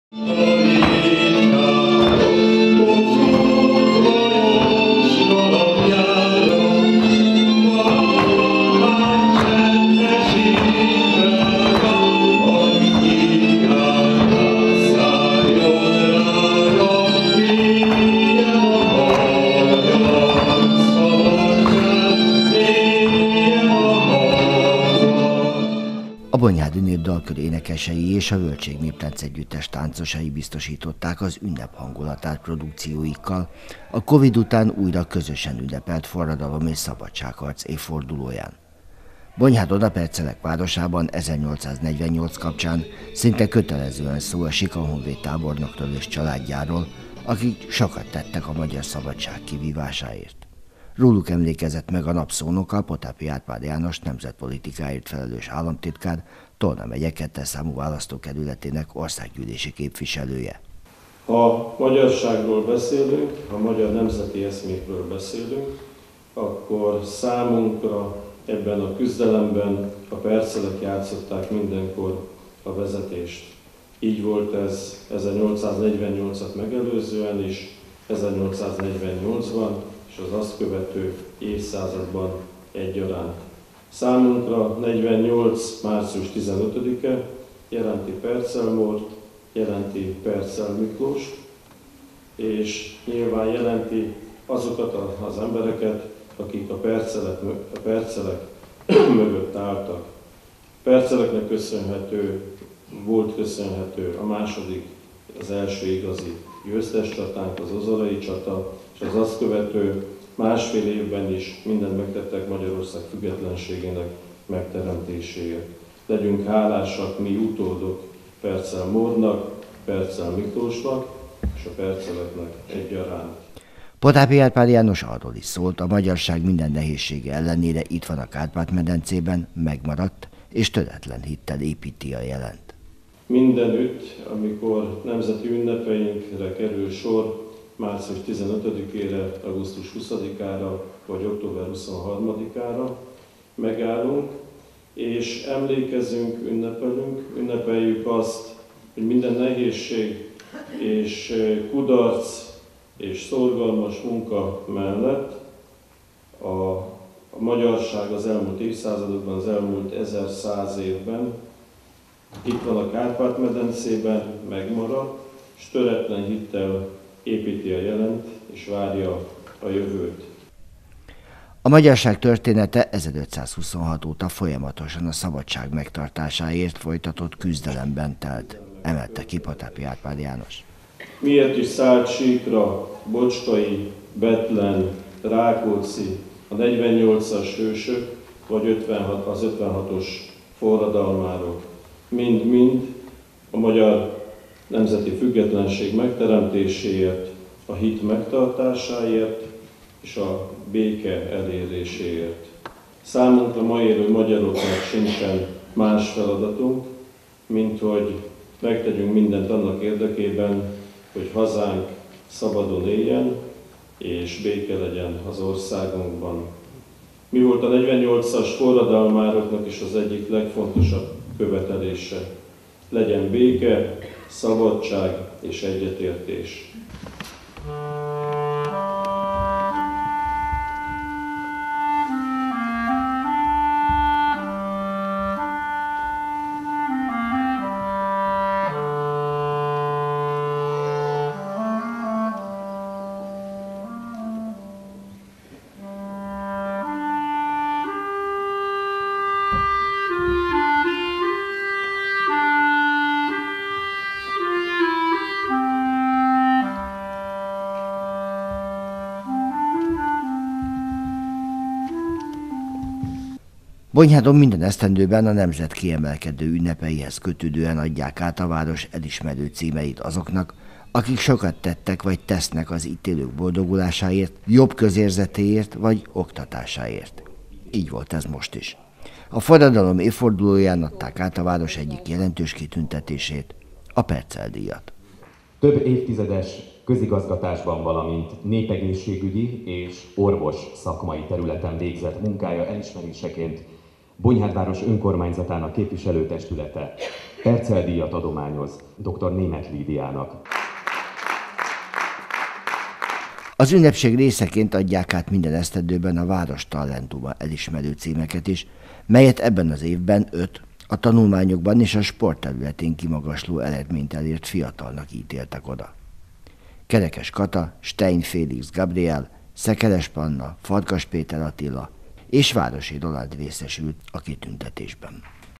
Ünnepség keretében emlékeztek meg a 1848-as forradalom eseményeiről
A szokásoknak megfelelően nagyszabású ünnepség keretében emlékeztek meg a 1848-as forradalom eseményeiről szerte az országban, így Bonyhádon is. A program keretében adták át a Perczel díjat valamint a Bonyhád talentuma kitüntető címeket, melyet idén öt ifjú tehetség érdemelt ki.